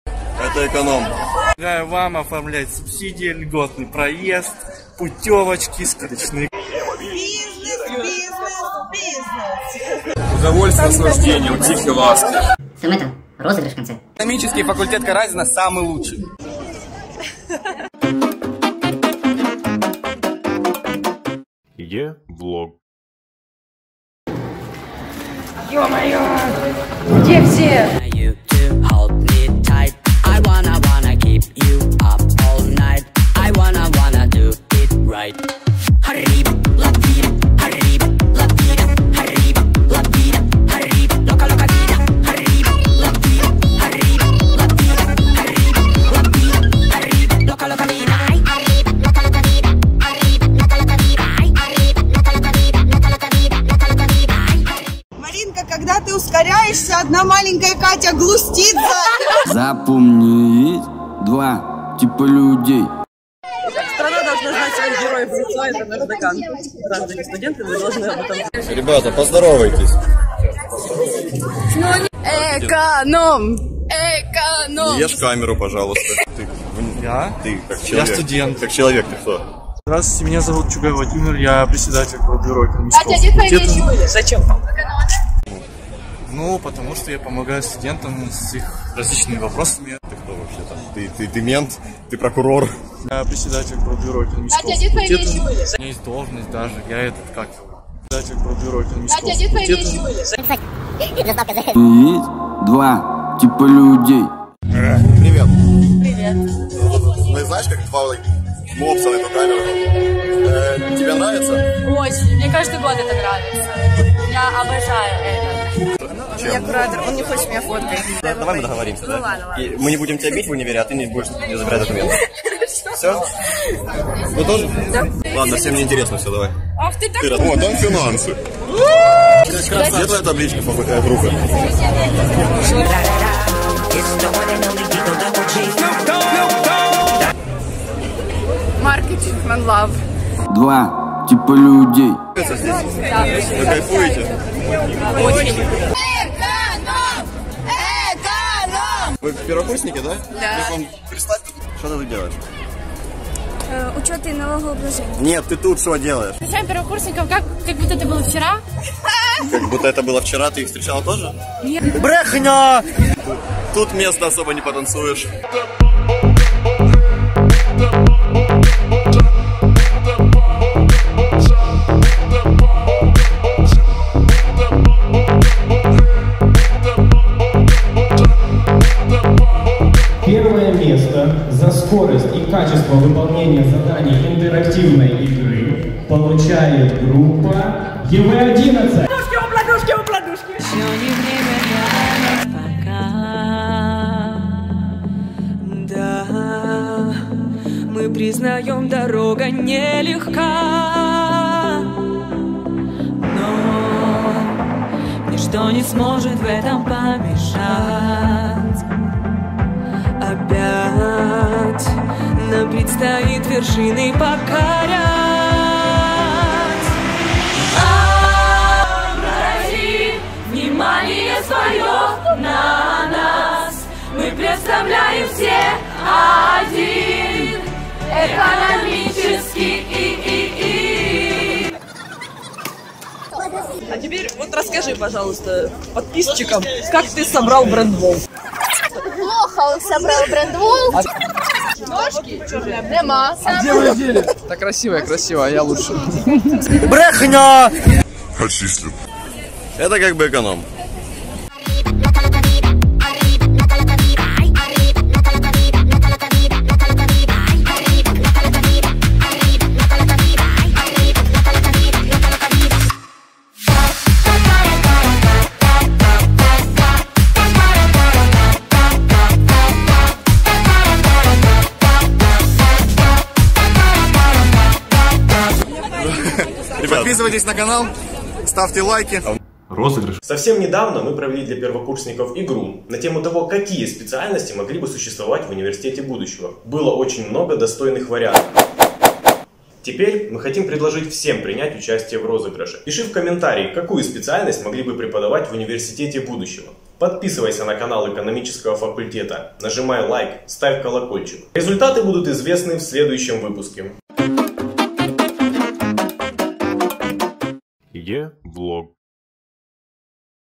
E-VLOG [Полётный Выпуск]
Экономический факультет ХНУ имени В.Н. Каразина подготовил для Вас уникальный формат влогов.
Дальше больше!

ВНИМАНИЕ РОЗЫГРЫШ!
Пиши в коментариях, какую специальность могли бы преподовать в университете будущего.
Победит самый креативный!

Результаты в следующем выпуске.